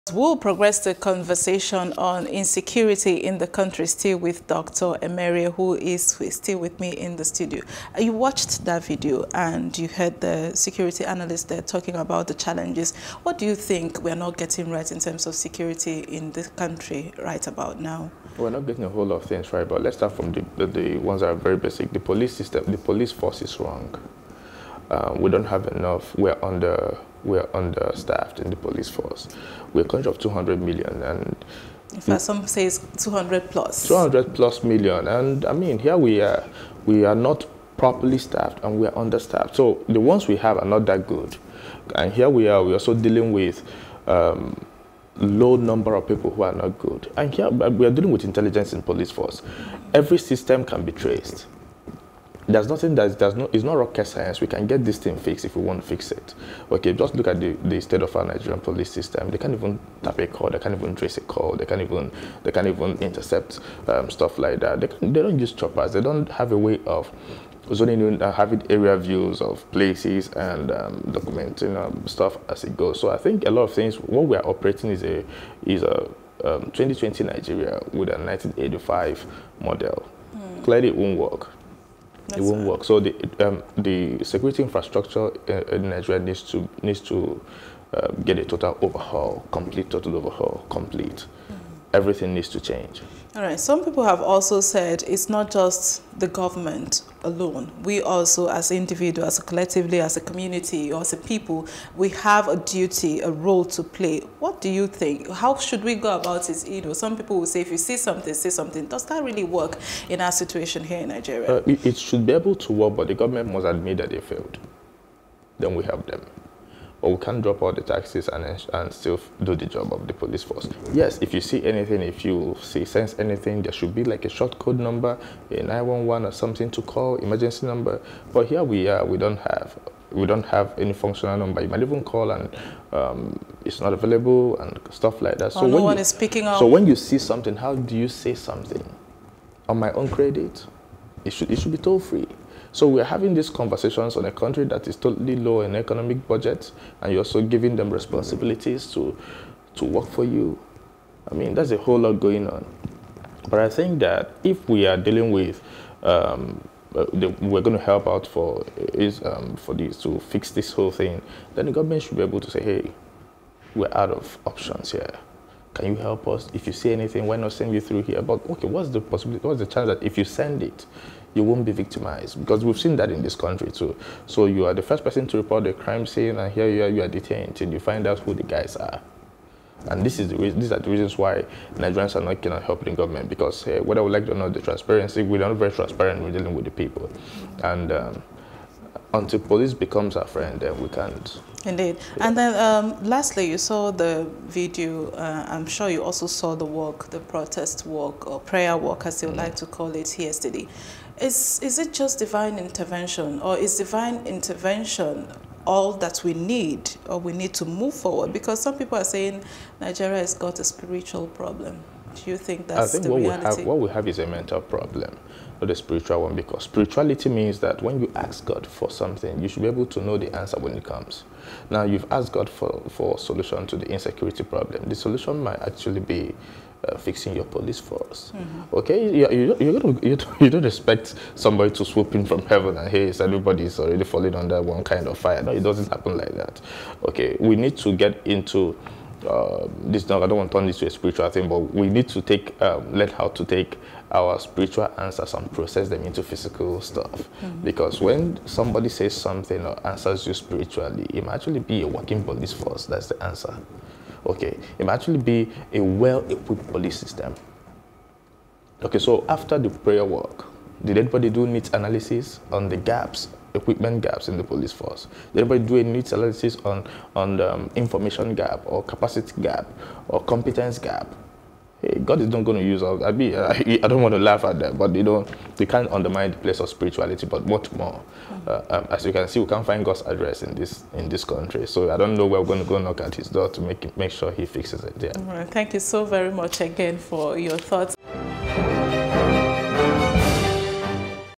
Expert: "Nigeria's Police Force Operate With 1985 Models In 2020"
General Overseer of the Redeemed Christian Church of God (RCCG), Enoch Adeboye, on Sunday, led members of his church in a prayer walk against insecurity in Nigeria.

Dr. Emerie Agunwa, public affairs analyst joined us in the studio.